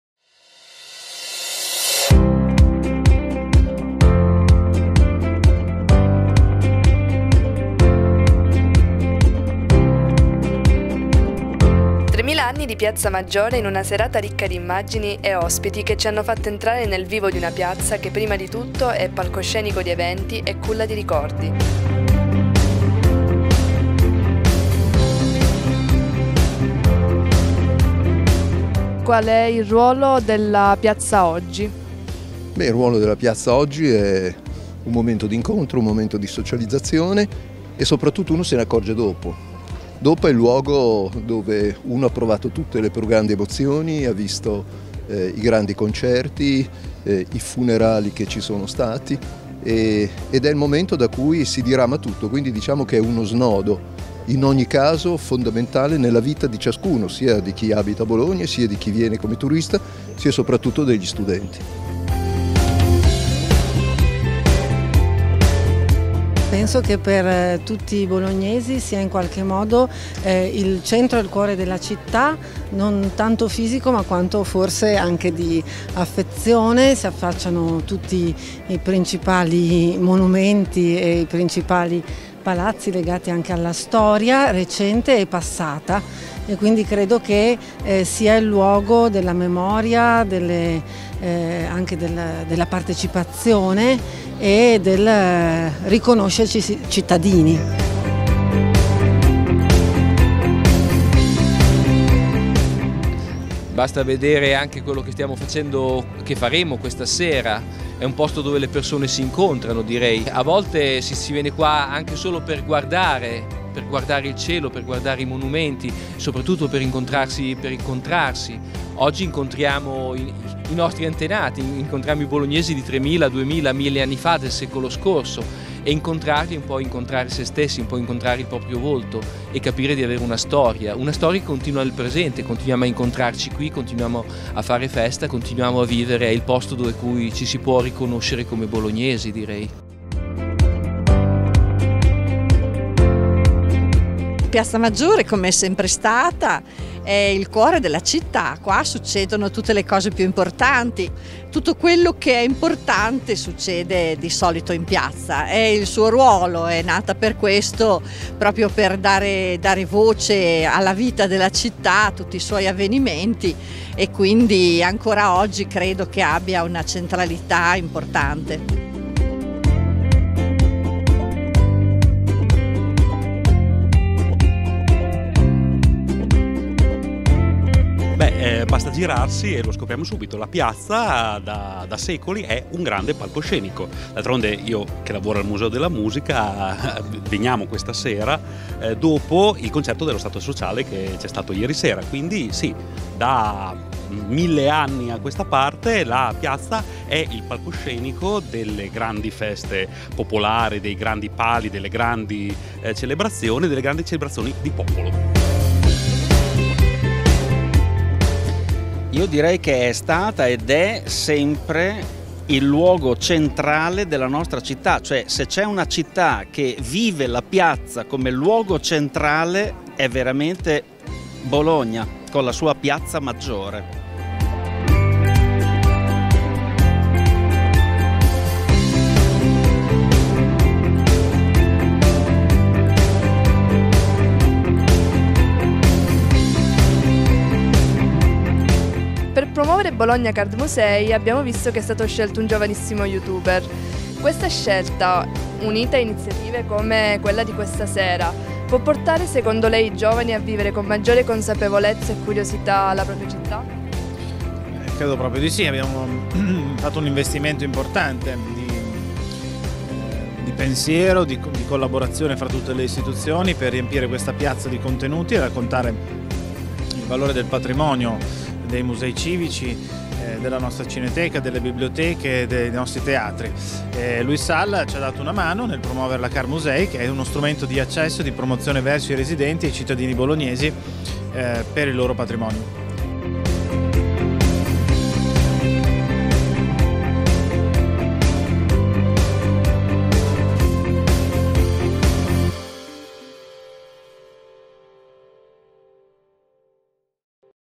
|| Bologna estate 2018 || Tremila anni di storia
Piazza Maggiore è stata raccontata da esperti professionisti che ci hanno condotto in un viaggio indietro nel tempo, illustrando la nostra amata piazza dal tempo degli Etruschi fino ad oggi.
La serata, introdotta dall'Assessore alla Cultura Matteo Lepore, è stata realizzata all'interno del programma di Bologna Estate 2018.